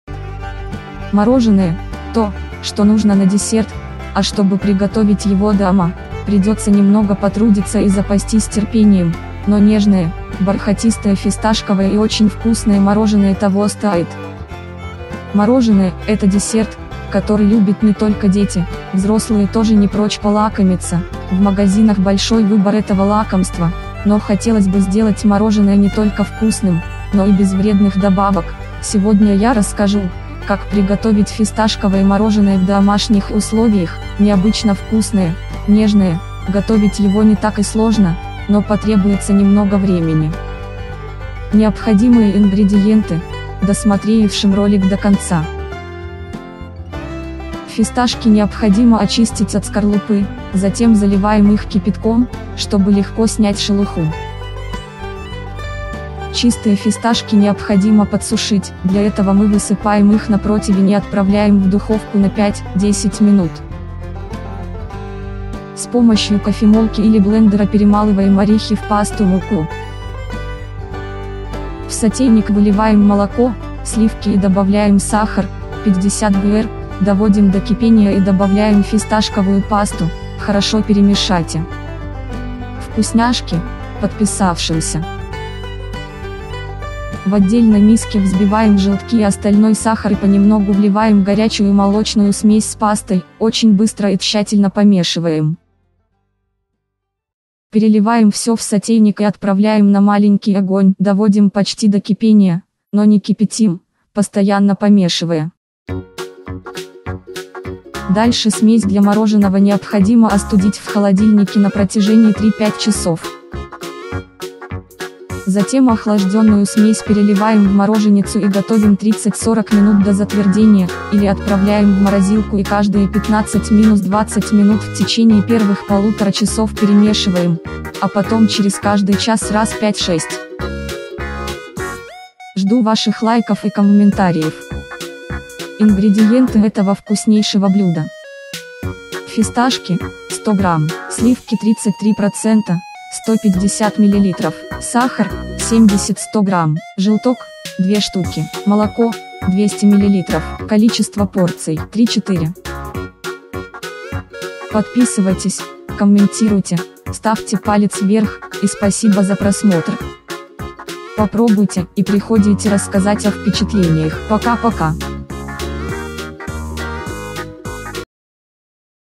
Фисташковое мороженое Рецепт, пошаговый, как приготовить, видеорецепт
Деликатес, доступный каждому! Мороженое — то, что нужно на десерт. А чтобы приготовить его дома, придется немного потрудится и запастись терпением. Но нежное, бархатистое фисташковое и очень вкусное мороженое того стоит.